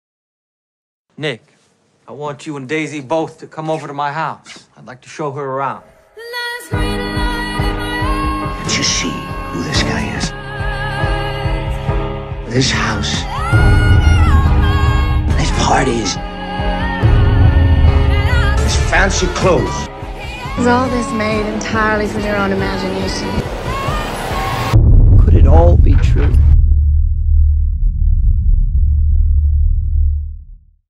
The Great Gatsby 30 second fan made
audio is rough cause there is so much background noise in what I took the footage from.  My spin on theTiffany Jewelry behind the scenes video